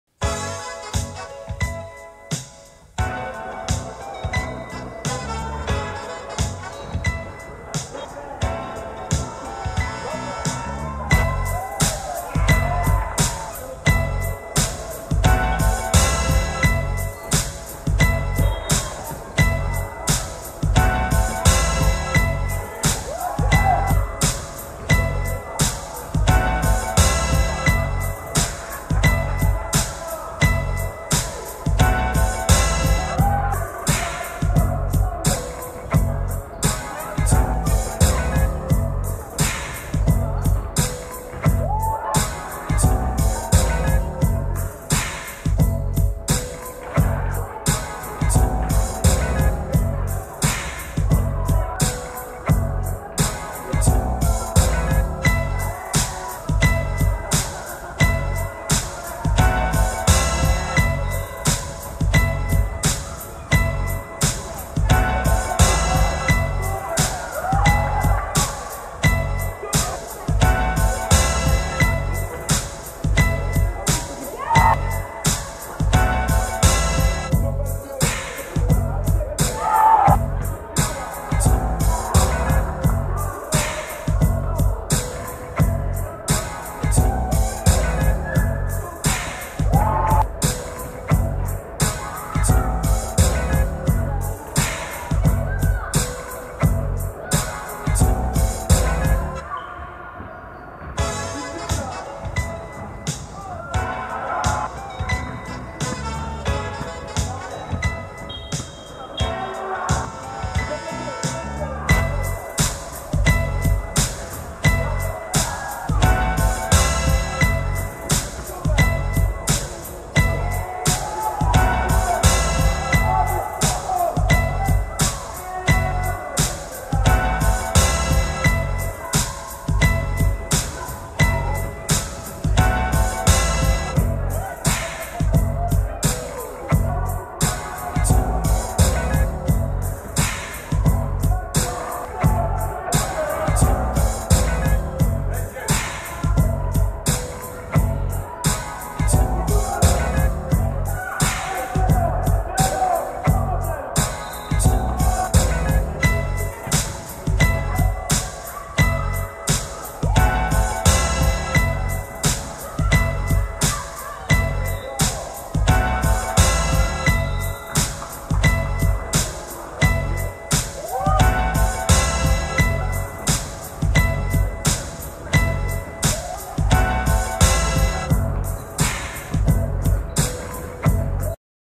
10 02 Wilki vs Sokoły – Dzika Liga 12
WILKI 44:73 SOKOŁY
D.Johnson 19pkt 7zb 6ast 5stl
Z.Adamczyk 12pkt 23zb 3ast 3stl